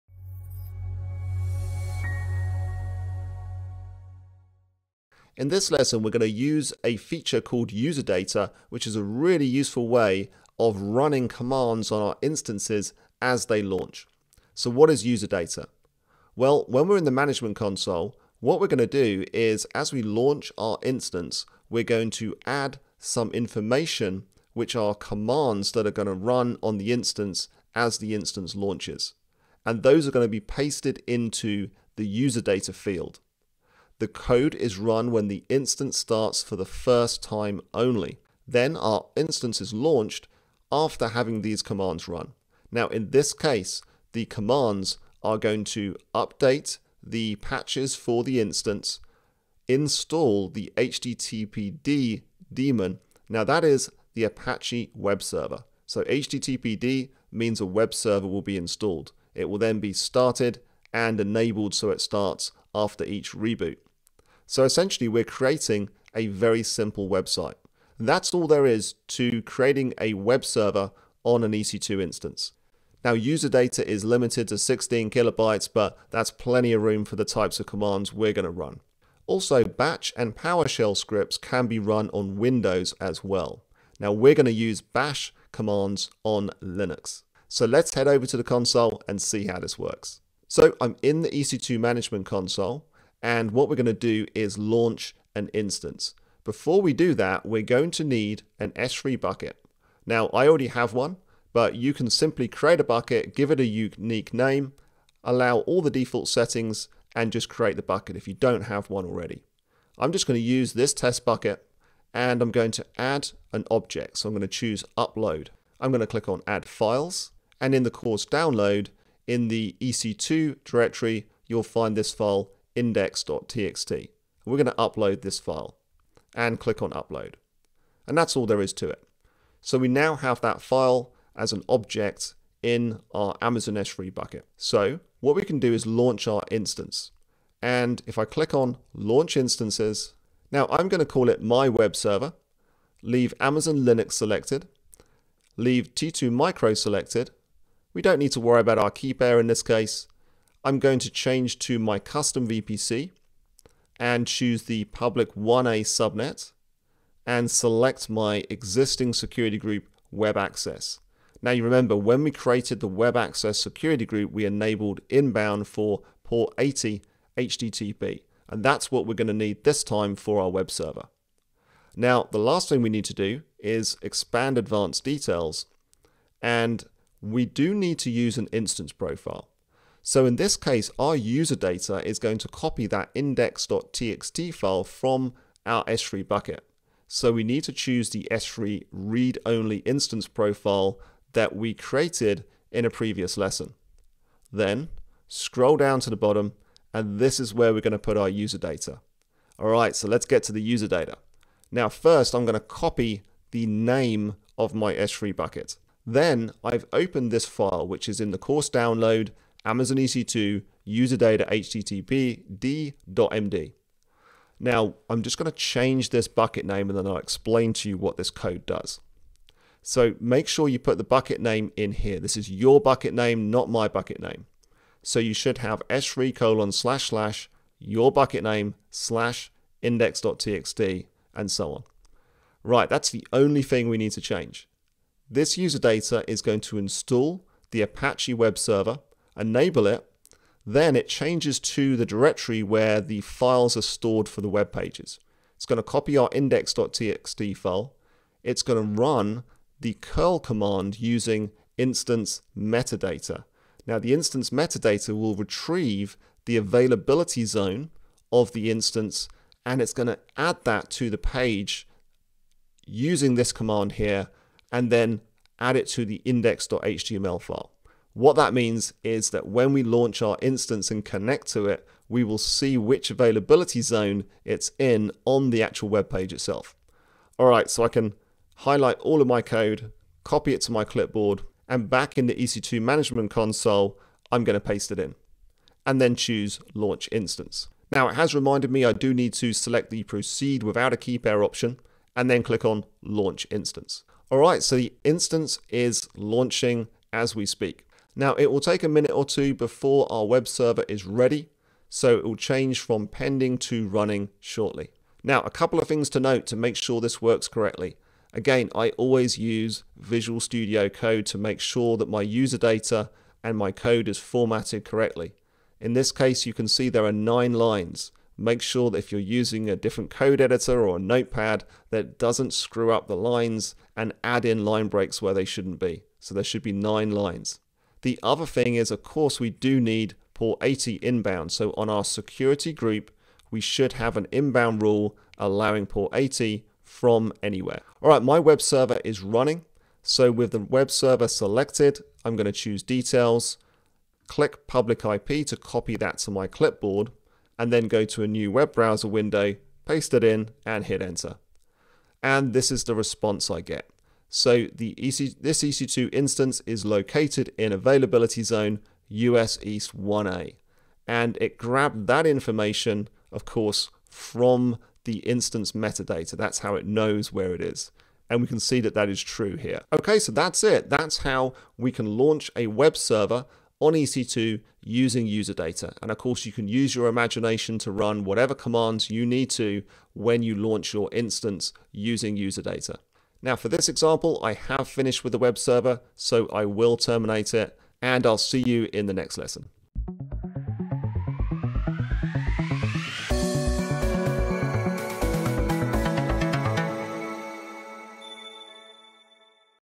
Create a Website with User Data
This AWS tutorial is from our Introduction to Cloud Computing on AWS for Beginners course which is ideal for those just getting started learning the AWS Cloud.

📌 Timestamps
0:00 - Introduction
1:30 - Preparing S3 Bucket
2:14 - Launching EC2 Instance
2:52 - Configuring User Data
3:55 - Launch Process
5:40 - Verification & Testing

You'll follow along and create an Apache website running on an Amazon EC2 instance in the AWS Cloud. This is super easy when using user data which is simply code you supply to the instance at launch time. Make sure you download the code file below: